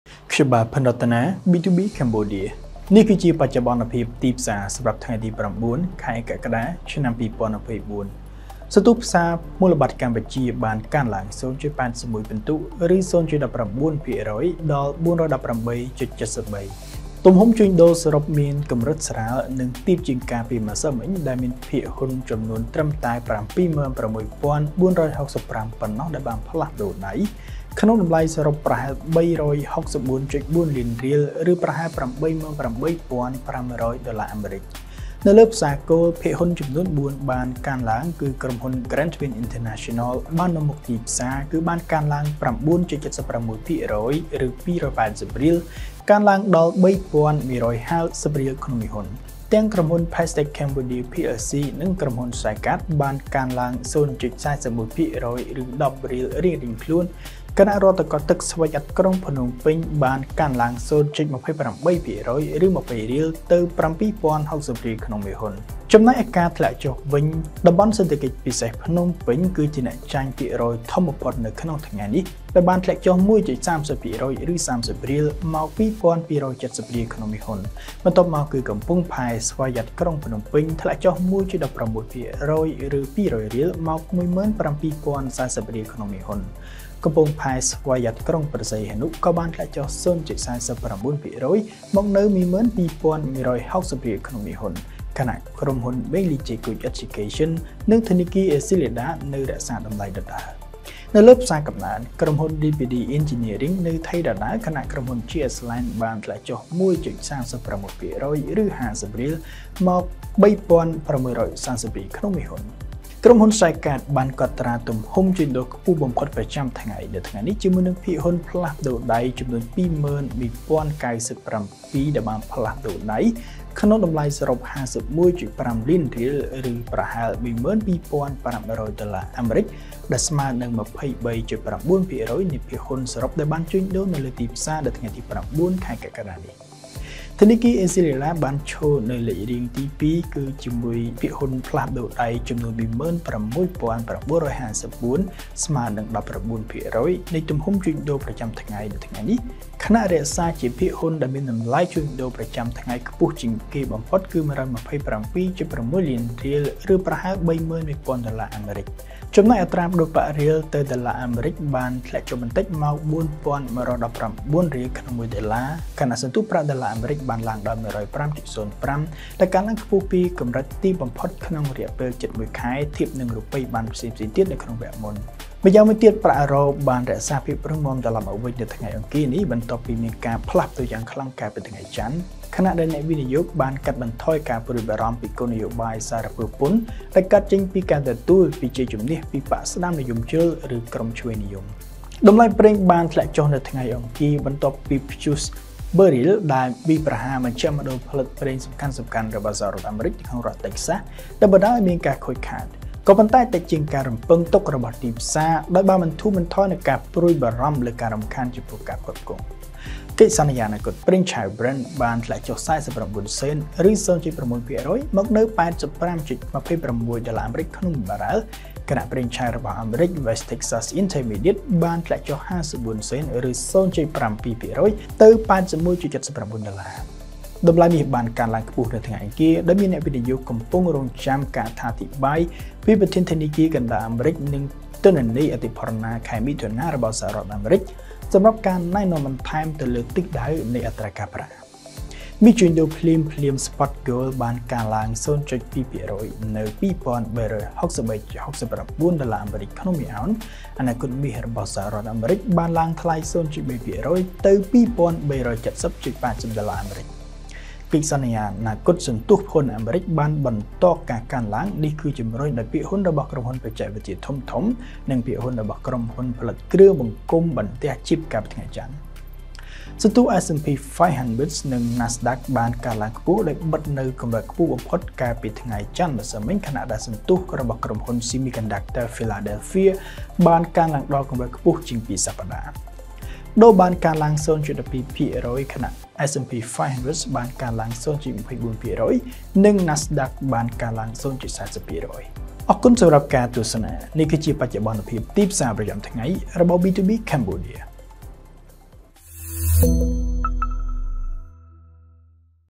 Market Update | បច្ចុប្បន្នភាពទីផ្សារភាគហ៊ុន (9 July, 2024)
B2B Cambodia នាំមកជូនលោកអ្នកនូវបច្ចុប្បន្នភាពទីផ្សារភាគហ៊ុនសម្រាប់ថ្ងៃទី៩ ខែកក្កដាឆ្នាំ២០២៤ ដោយពិនិត្យមើលទីផ្សារមូលបត្រកម្ពុជា (CSX) និងទីផ្សារសកលជាភាសាខ្មែរ។

B2B Cambodia brings you a Market Update for 9 July, 2024, reviewing the Cambodia Securities Exchange (CSX) and global markets in Khmer.